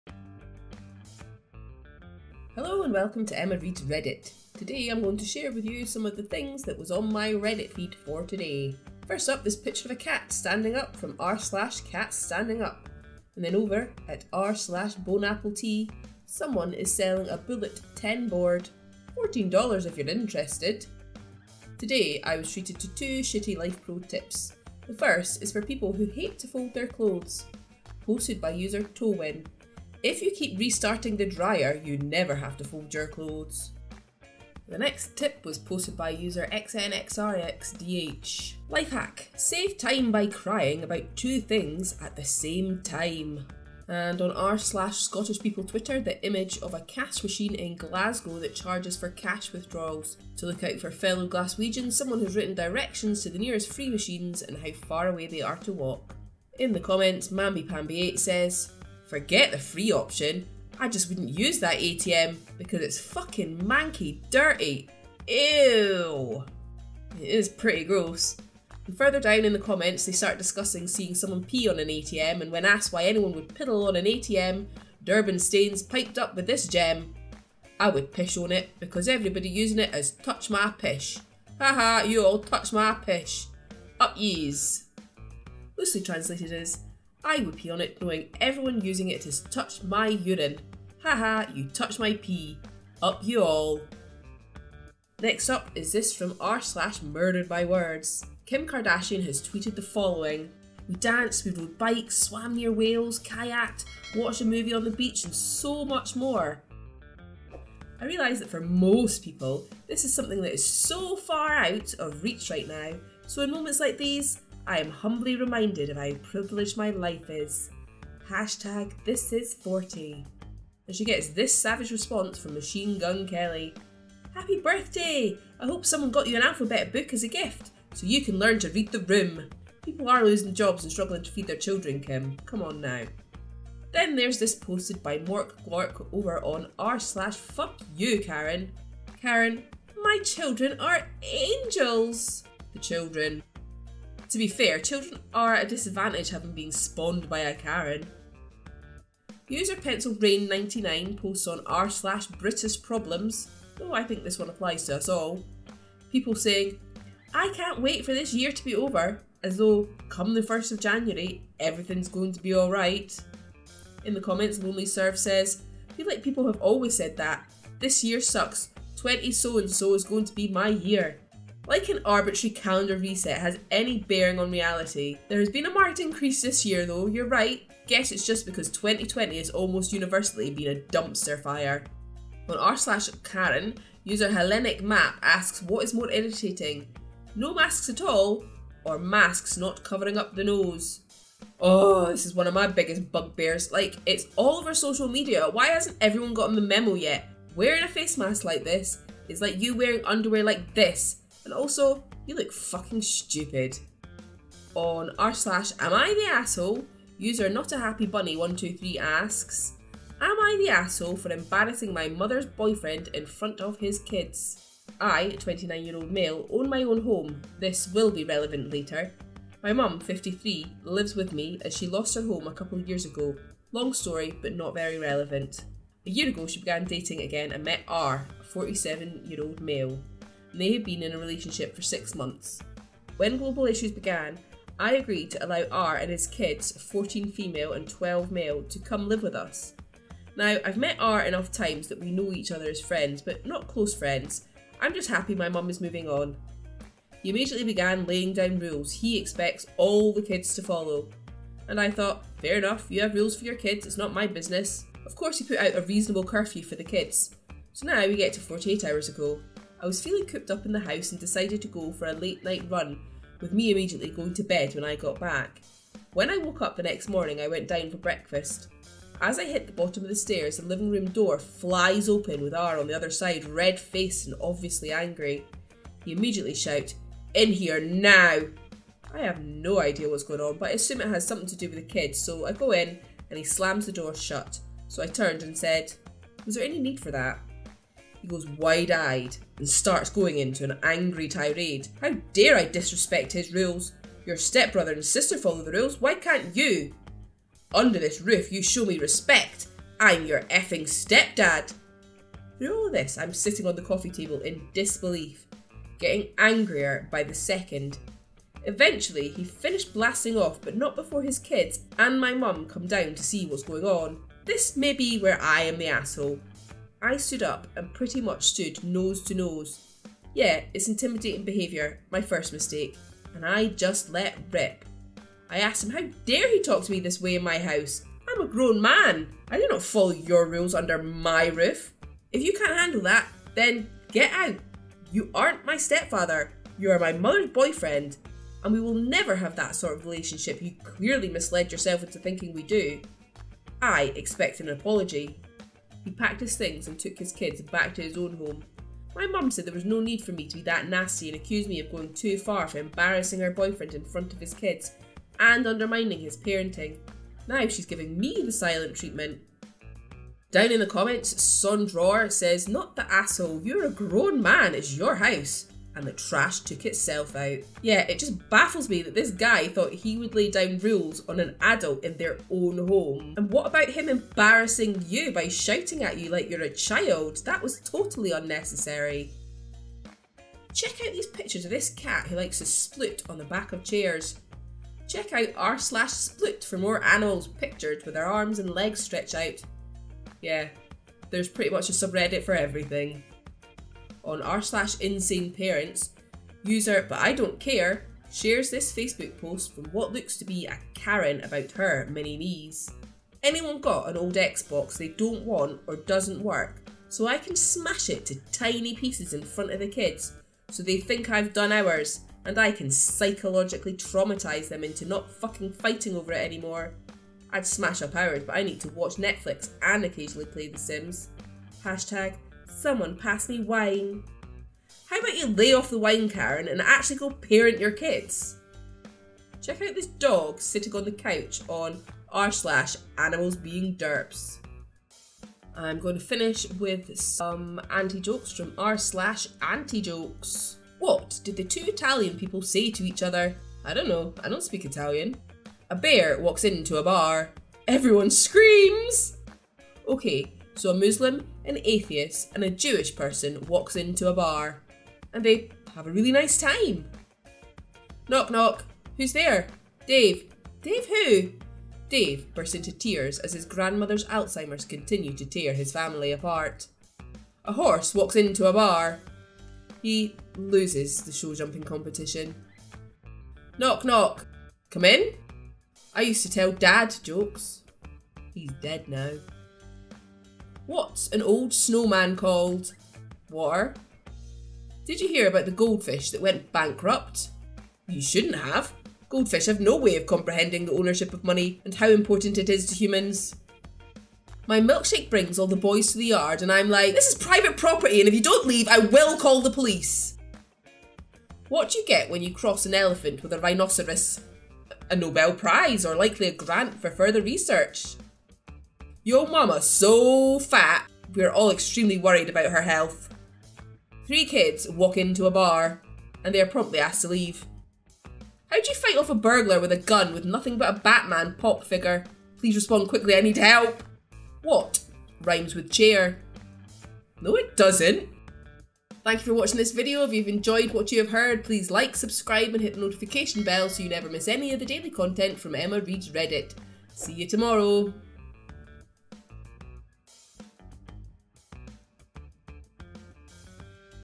Today on Reddit
Today I went through some of the posts on my reddit homepage.